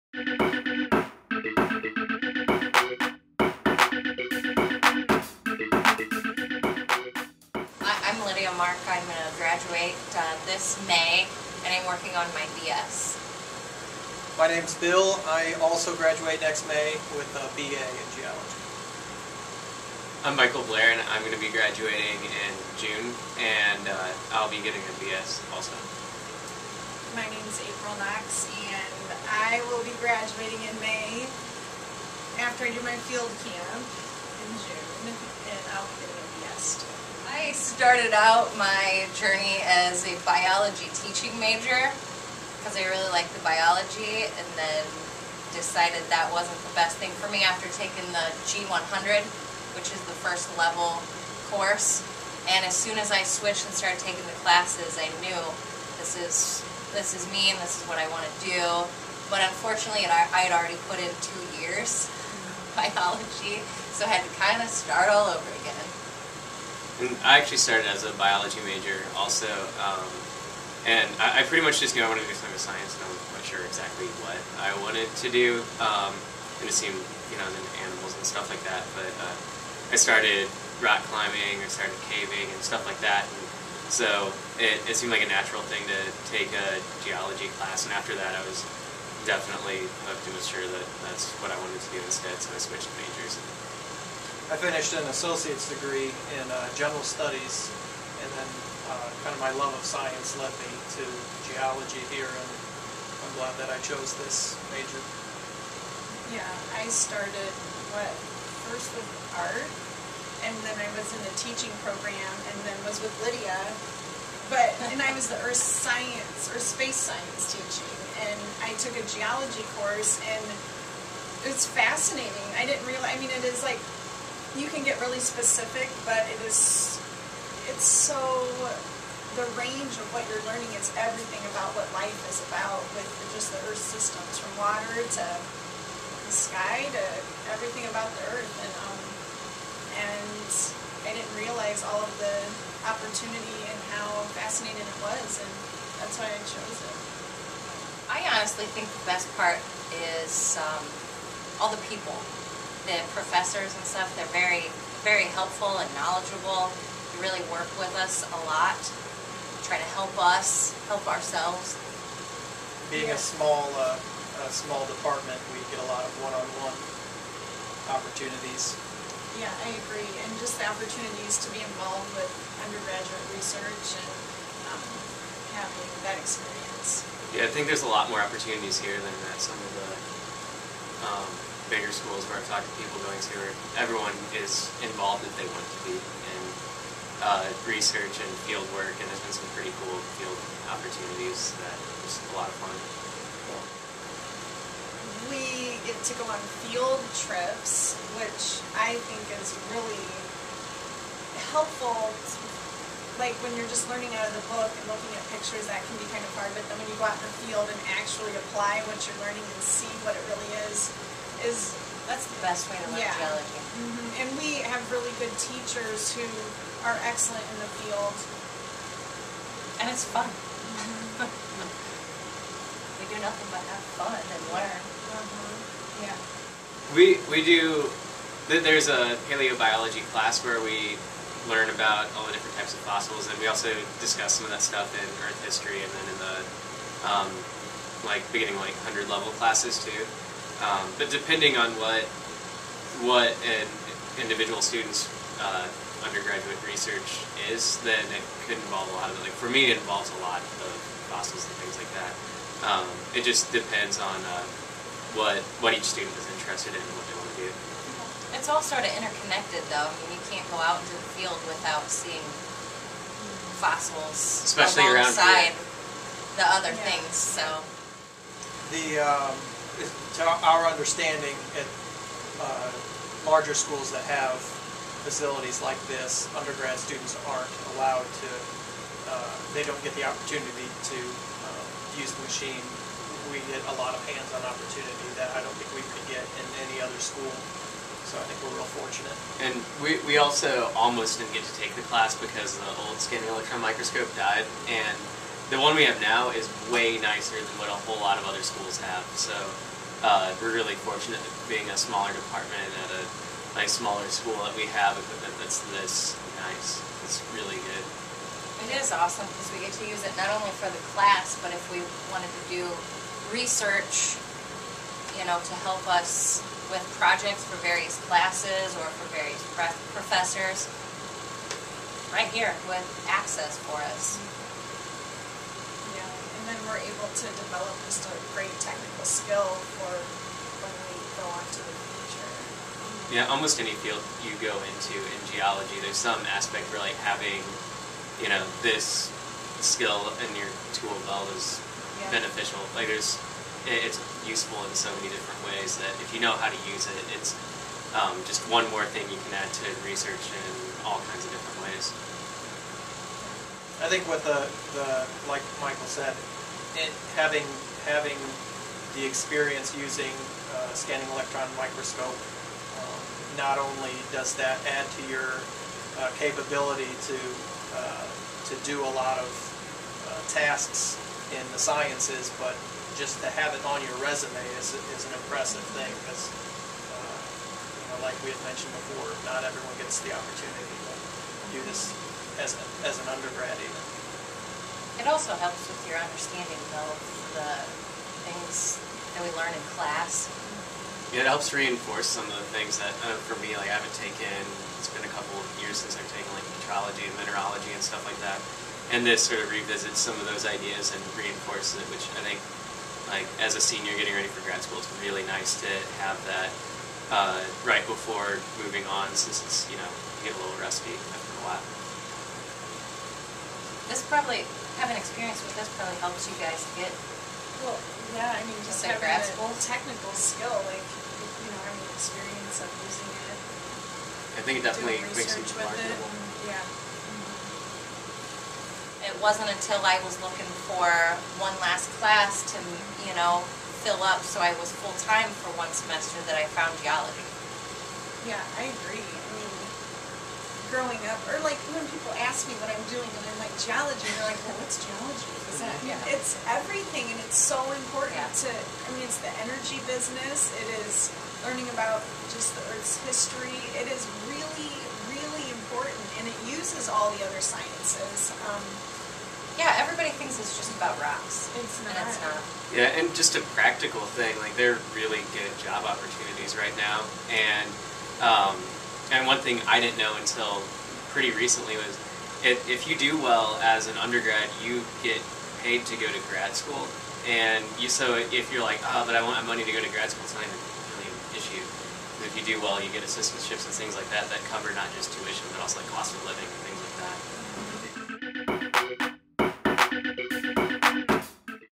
The Major Factor - Geosciences Students
Lydia Mark, Bill Ward, Michael Blair, and April Knox, geosciences students at IPFW, talk about how and why they chose their major, and what the great opportunities they have while an undergraduate student.